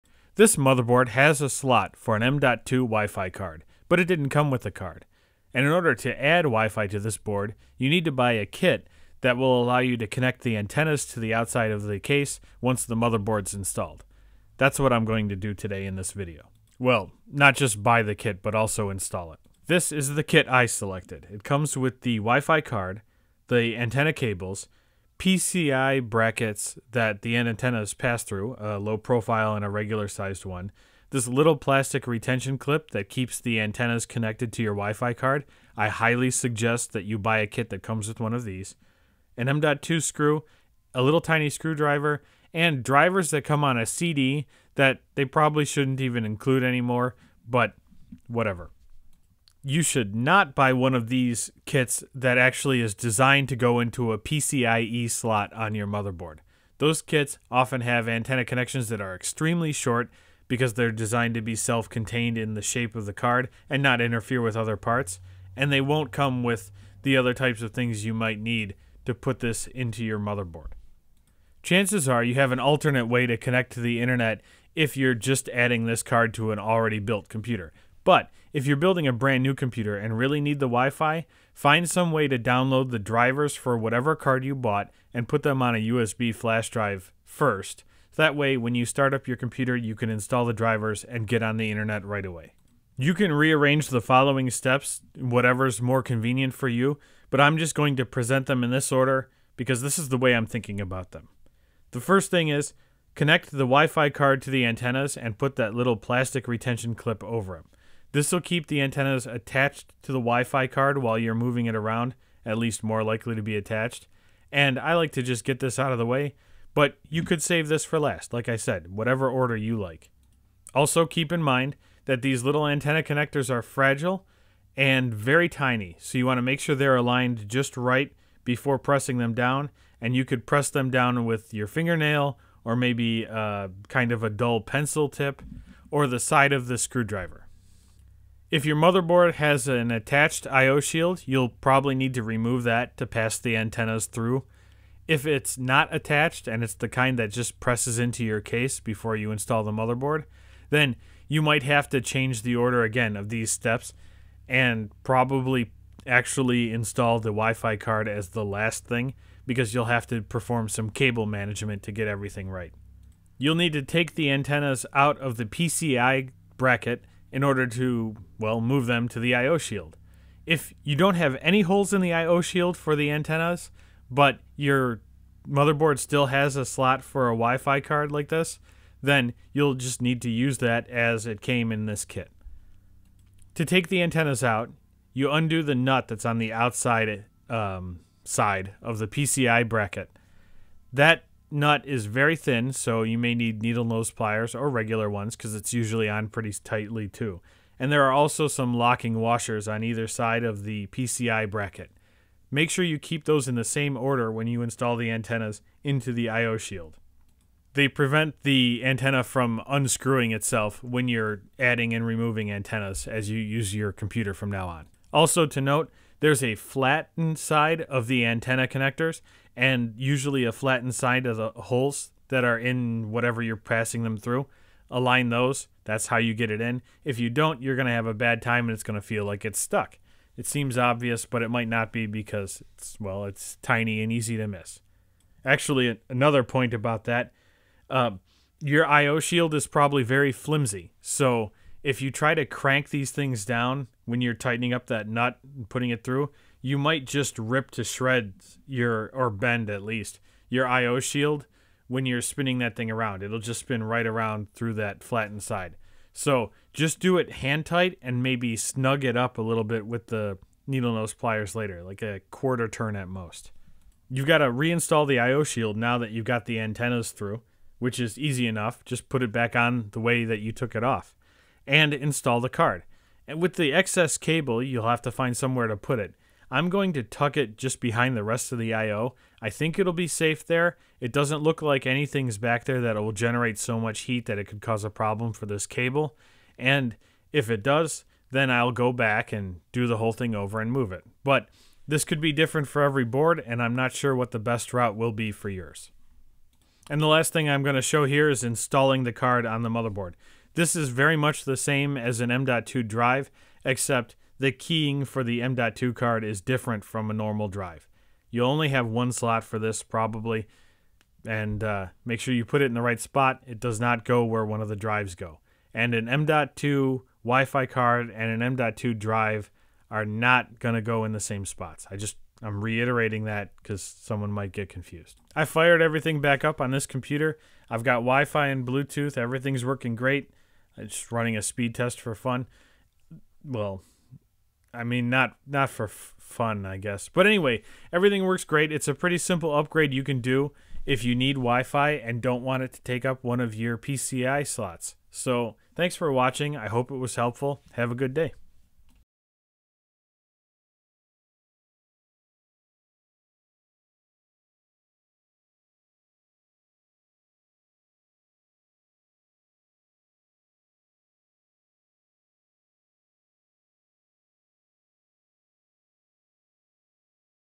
Chill Upgrade: Adding Wi-Fi to your Motherboard
My ASRock B650M-HDV/M.2 has a slot to add a Wi-Fi card, and has holes in the I/O shield for antennas, but didn't come with Wi-Fi. So, I had to add it and this video will show you the process to do that. At least, it should work for about 80% of you.

What I bought for this video was similar to this kit

Also, please support your local shelters, food banks, etc!